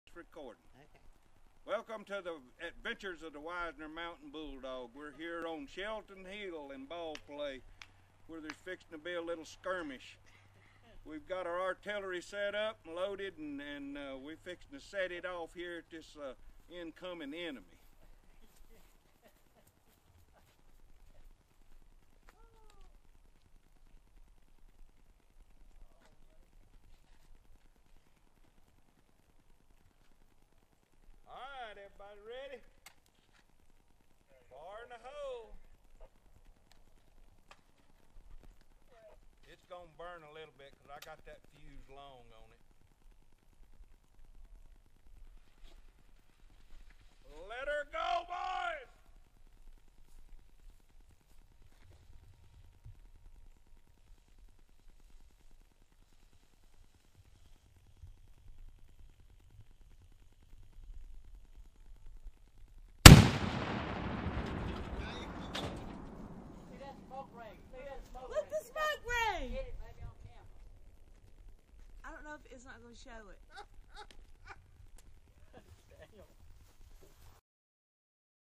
Shooting the old six pounder
Firing a 6 pound Napoleon Cavalry Cannon